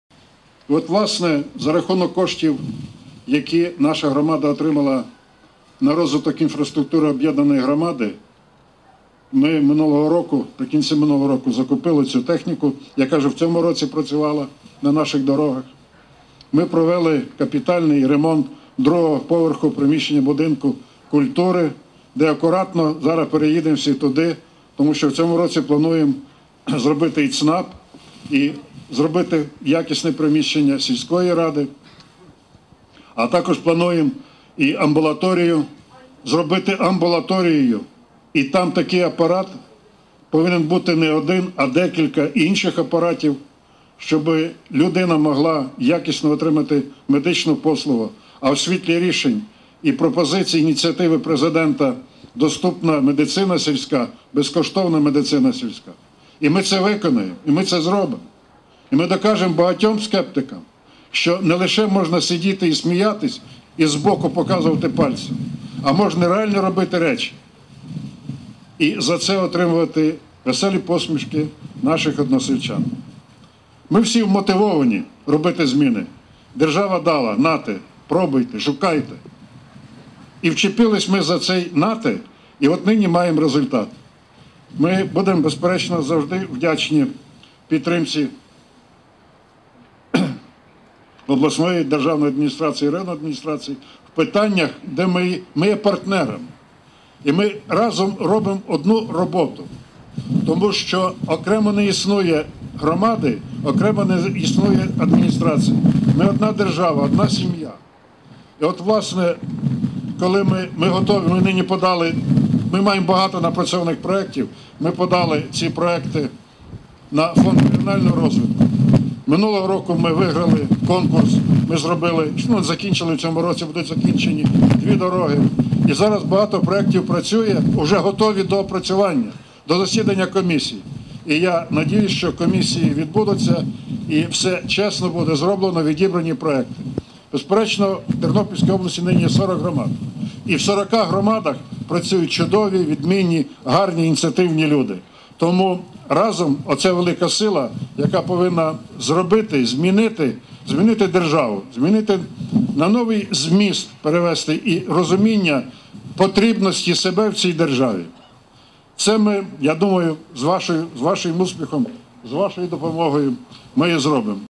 Байковецька ОТГ покращує інфраструктуру завдяки державній субвенції
У Тернопільській області об’єднані територіальні громади активно працюють над покращенням інфраструктури та рівня життя місцевих мешканців. Прикладом успішної та вмотивованої стала Байковецька ОТГ. За рахунок субвенції на формування інфраструктури там придбали 5 одиниць техніки, ключі від якої 29 січня вручив голова Тернопільської ОДА Степан Барна.

«За рахунок коштів, які наша громада отримала на розвиток інфраструктури, ми наприкінці минулого року закупила цю техніку, яка вже у цьому році працювала на наших дорогах. Ми провели капітальний ремонт другого поверху приміщення будинку культури. Плануємо створити ЦНАП та якісно оновити приміщення сільської ради, також оновити амбулаторію, щоб кожен мешканець зміг отримати якісні медичні послуги. Ми це виконаємо і доведемо багатьом скептикам, що можна робити реальні речі і за це отримувати веселі усмішки односельчан. Ми всі вмотивовані робити зміни, адже держава дала нам для цього ресурси, якими ми скористалися», - зазначив голова Байковецької ОТГ Анатолій Кулик.